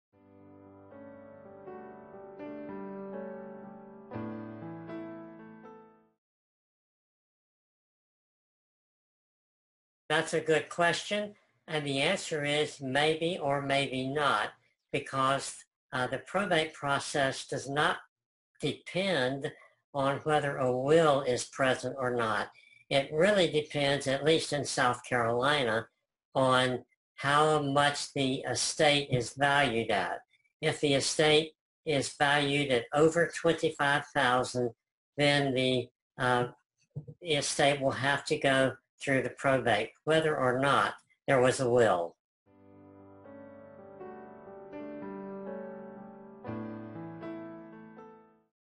Does A Person Have To Go Through Probate If He Doesn’t Have A Will?
The Greene Law Firm, P.A.
11 McGee Street
Greenville, SC 29601
United States

It depends on circumstances if a person have to go through probate if he doesn’t have a will.